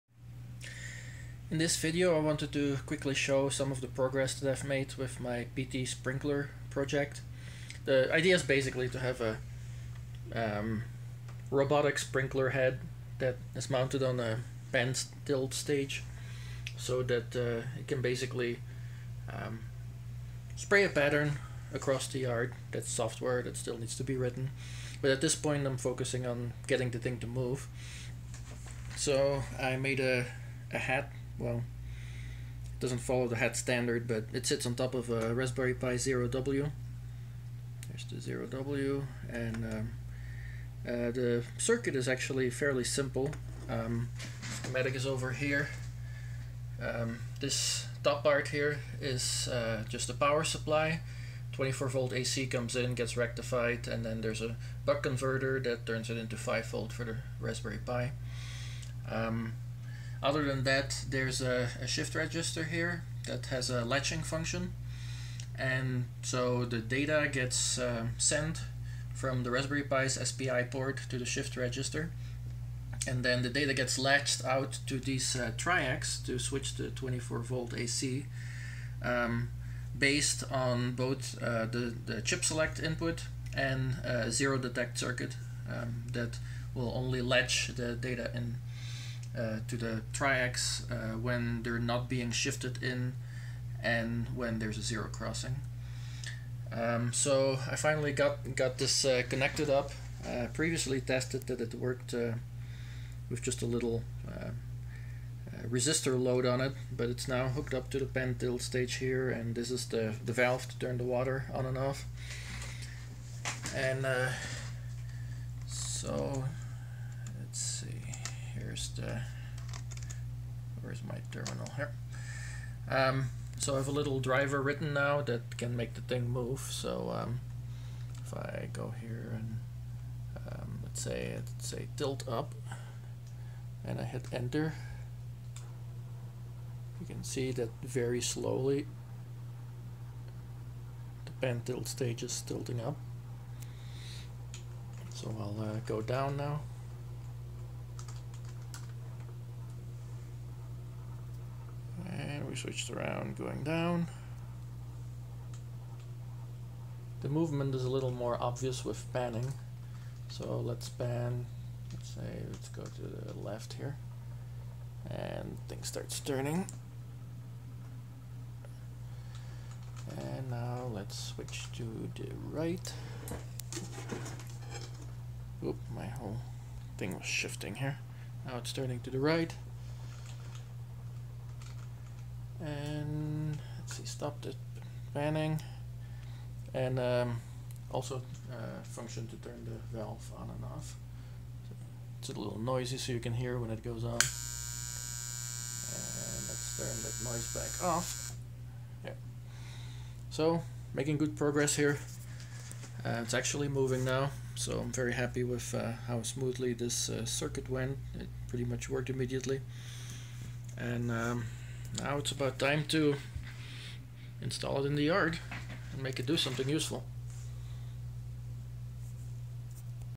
PTSprinkler project - first movement
First test of the PTSprinkler project with the Raspberry Pi based driver board controlling the pan/tilt stage and valve from Python.